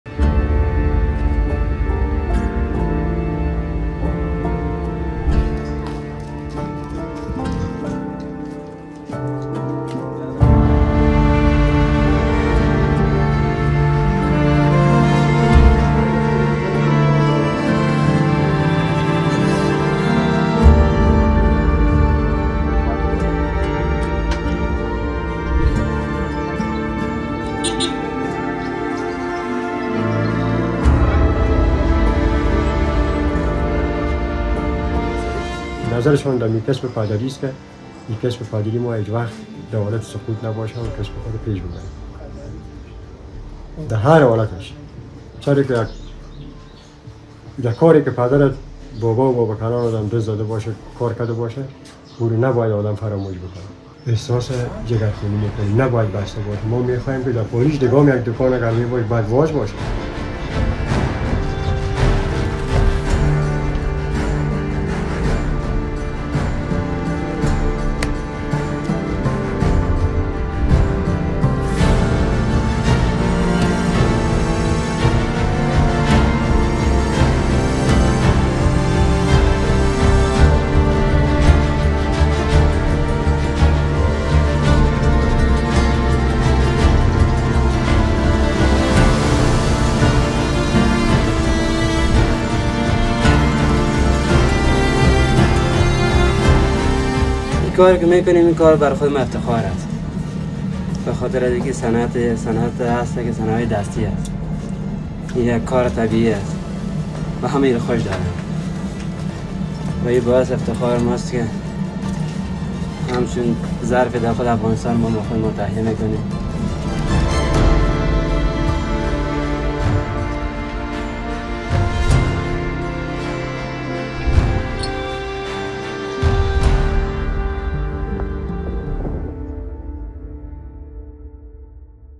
حرفيو الفخار يصارعون لإبقاء مهنتهم في قرية "كولالان" الأفغانية
مع إغلاق كثيرين محالهم.. حرفيو الفخار يصارعون لإبقاء مهنتهم على قيد الحياة في قرية "كولالان" الأفغانية رغم تردي الأوضاع الاقتصادية وقلة حركة البيع
لمعرفة أحدث المستجدات.. عبر البث المباشر من هنا ⤵
وبإمكانكم متابعة العربية على منصات التواصل